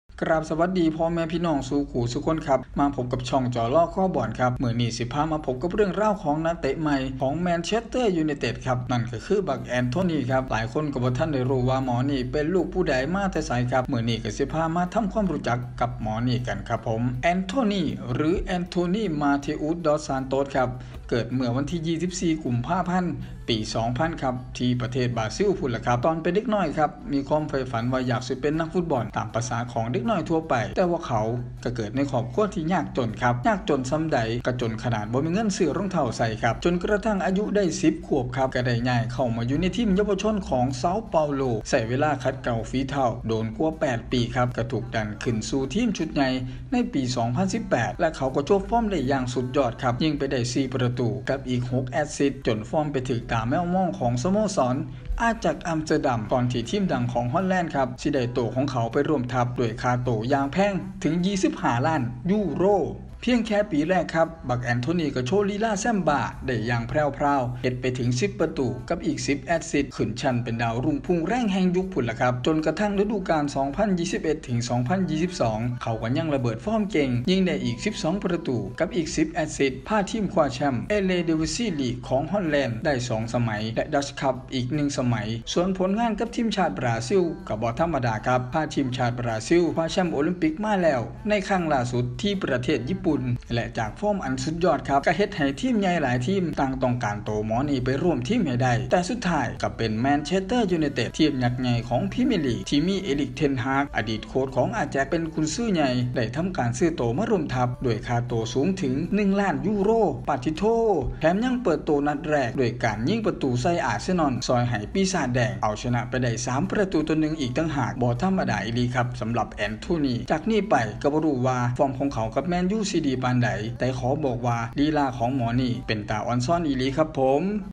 แอนโทนี่ เขาเป็นใครมาจากไหน แอนโทนี่ แมนยูไนเต็ด พรีเมียร์ลีก(พากย์อีสาน)
แอนโทนี่ Antony Matheus dos Santos